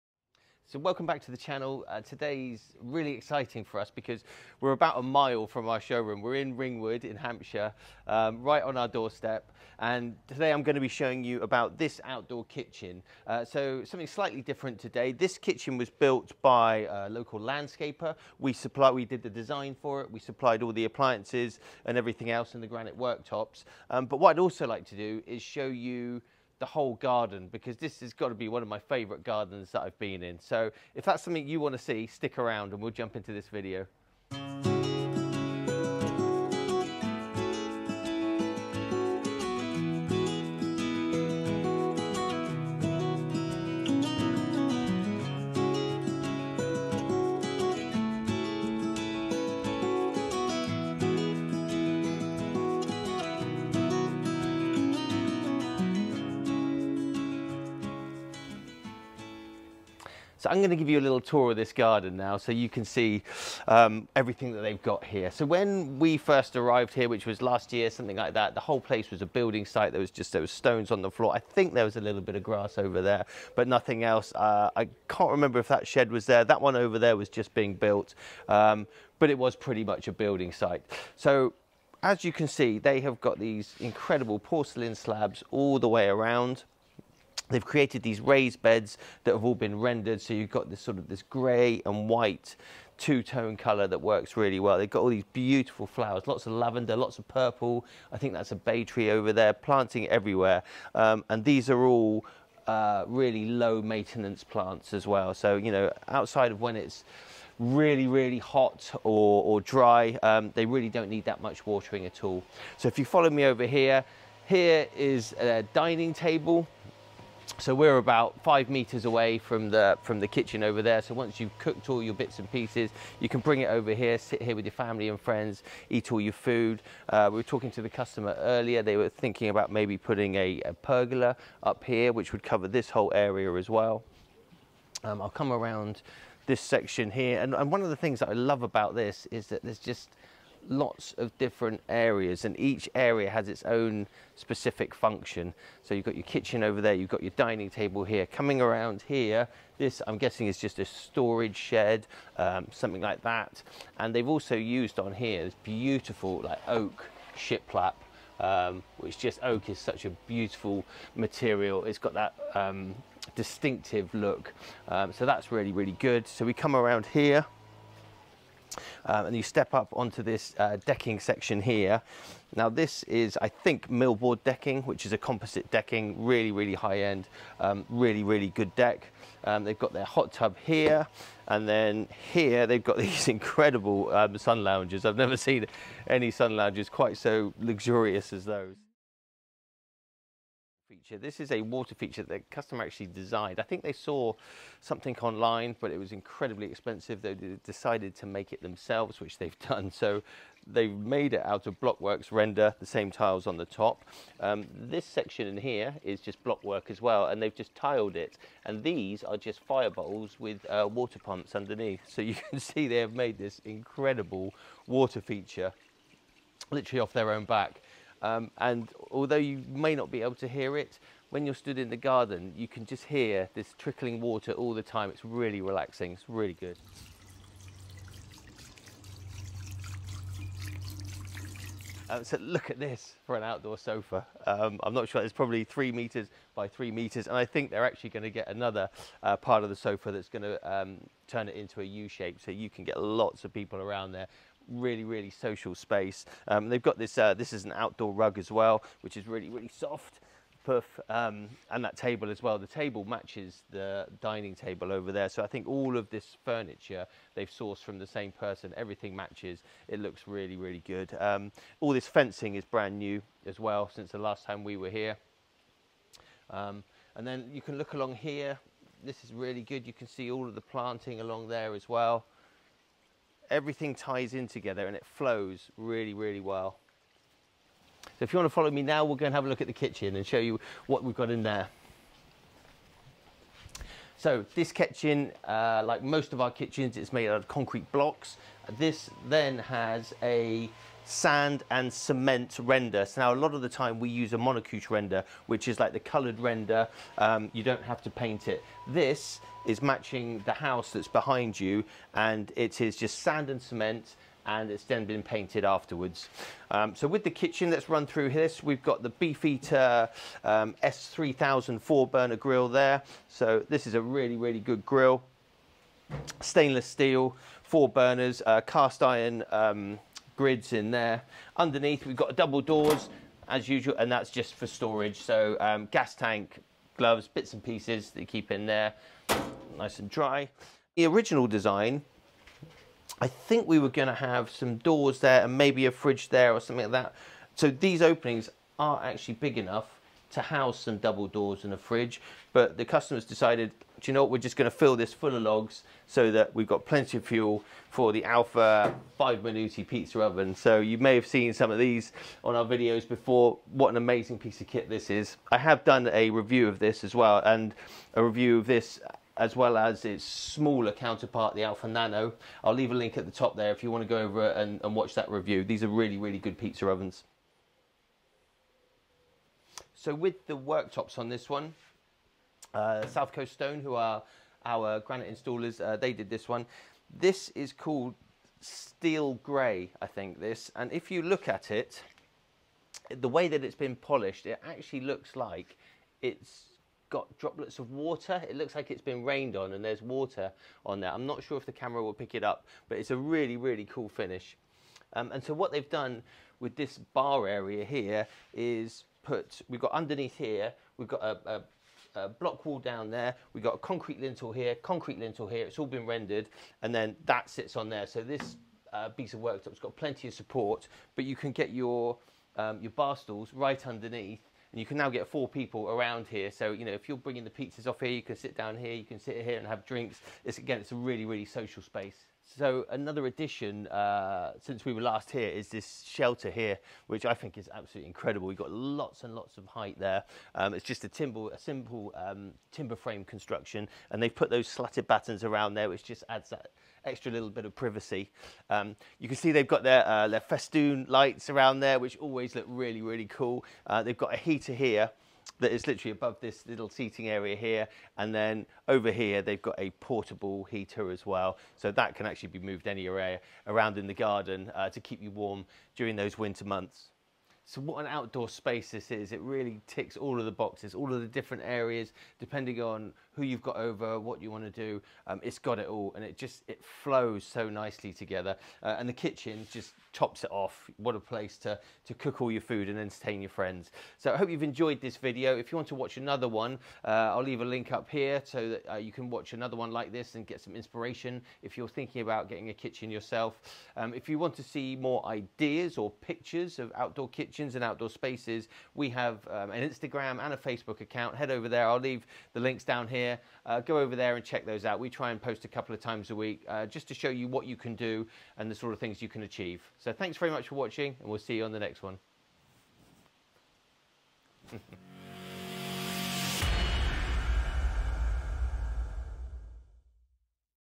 DIY OUTDOOR KITCHEN + ULTIMATE GARDEN TOUR | ALFA 5 MINIUTI | BEEFEATER BBQ
Join us on a tour of a garden you have always dreamed of having! And… of course… an Outdoor Kitchen built by us!

❗*Beefeater s3000- discontinued product*❗